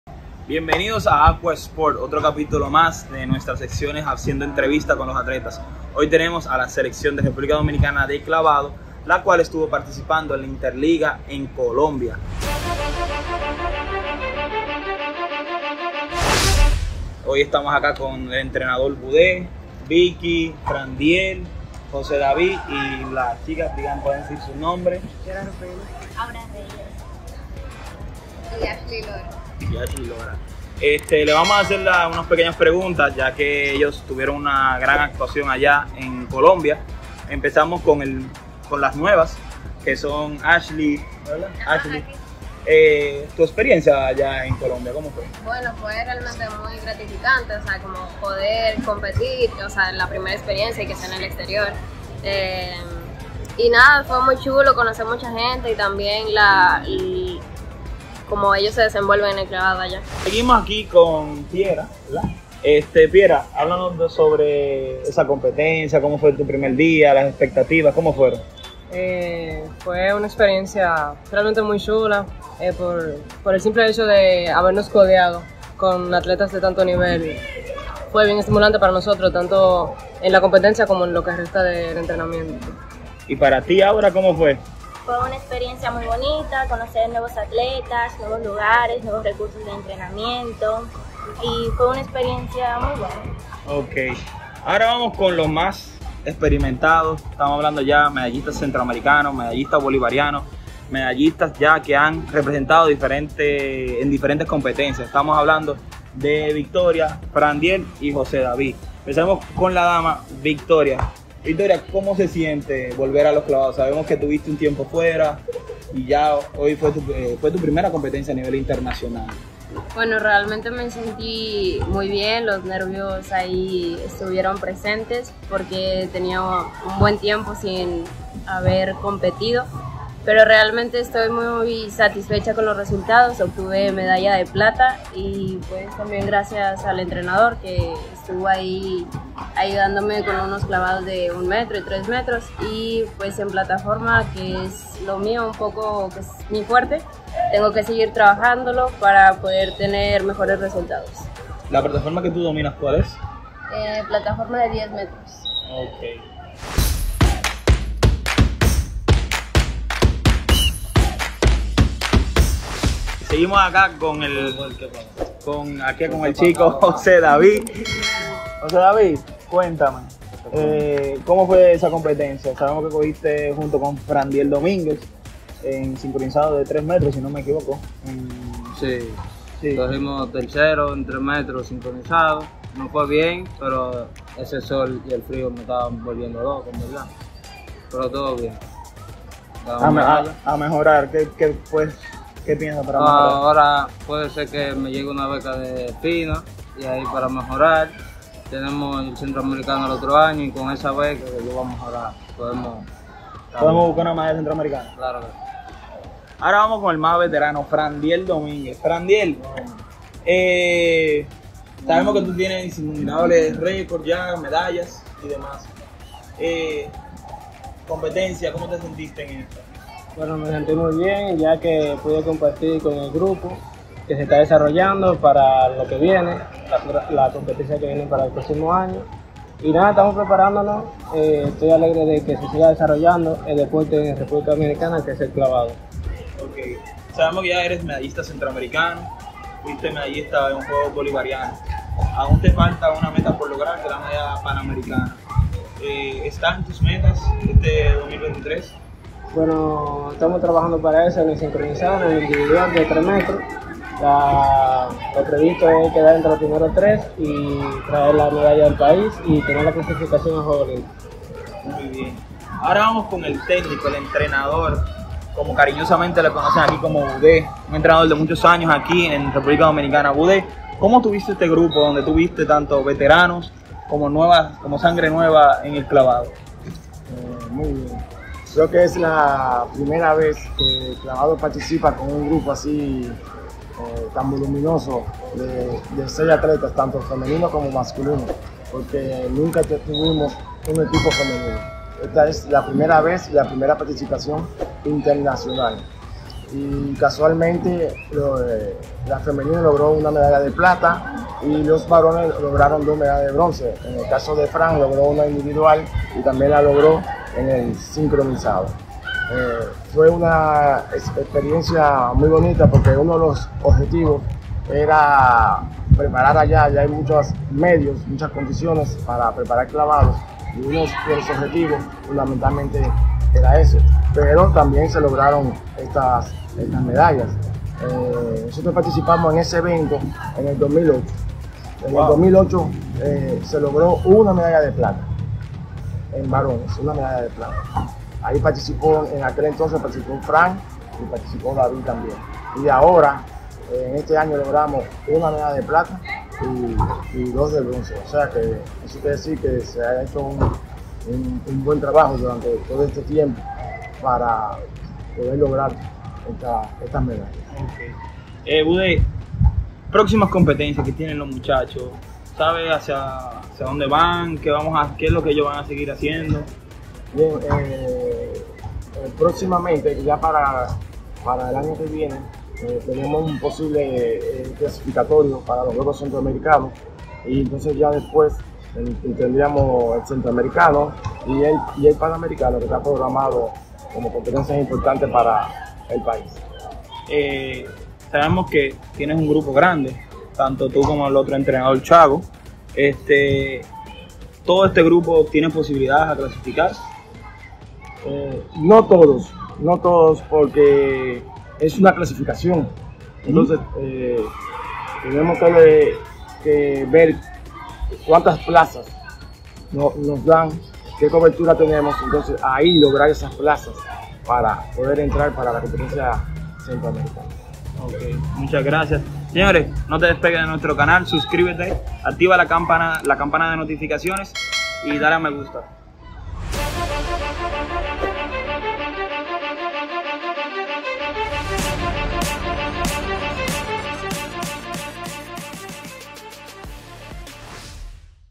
Rep.Dom logra excelente resultado en Colombia
Los Clavadista de Rep.Dom lograron una excelente participacion en el Inter ligas de Clavado realizado por  Federacion Colombiana Natacion, escuchemos lo que los atletas nos cuentan